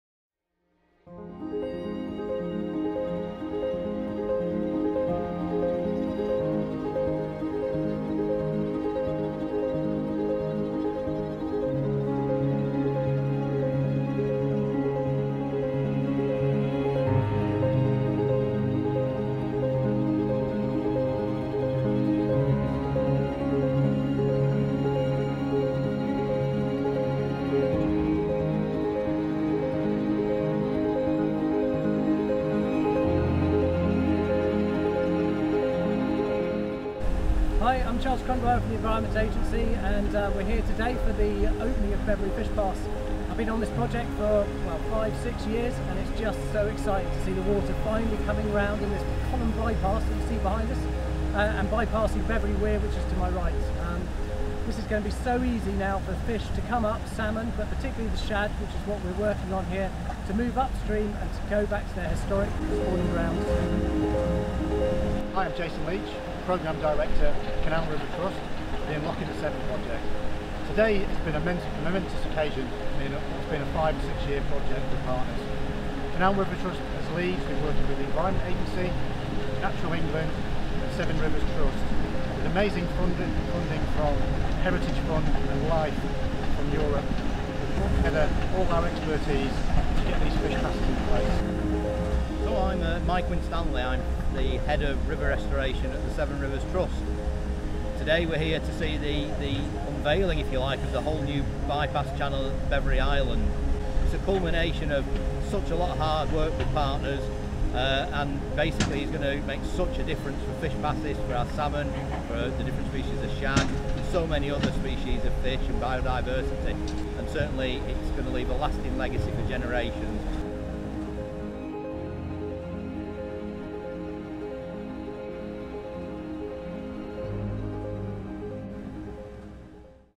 Bevere Fish Pass Opening - Unlocking the Severn September 2020
In this video we see the completed natural-style by pass channel at Bevere opened to allow the river water through. For the first time in more than 170 years, river fish - including the rare twaite shad - now have free passage around Bevere weir.